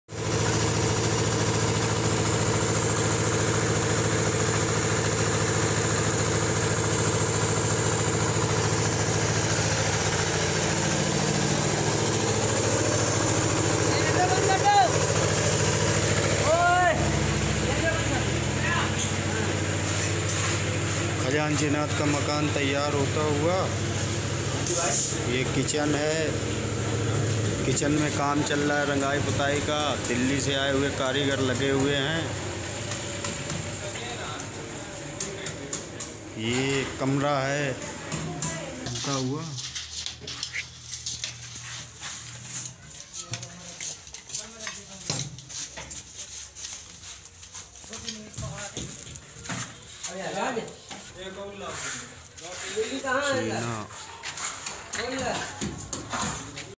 खजांची का जन्मदिन मनाने आज कानपुर देहात पहुंचेंगे अखिलेश यादव
कानपुर देहात : समाजवादी पार्टी के राष्ट्रीय अध्यक्ष अखिलेश यादव आज कानपुर देहात के सरदारपुर डेरा गांव पहुचेंगे. वे यहां खजांची का जन्मदिन मनाने पहुंच रहे हैं. अखिलेश यादव ने खजांची के परिवार को दो घर उपहार में दिए हैं, जो बनकर तैयार हो चुके हैं. अखिलेश के साथ बड़ी संख्या में पार्टी के नेता और कार्यकर्ता मौजूद रहेंगे.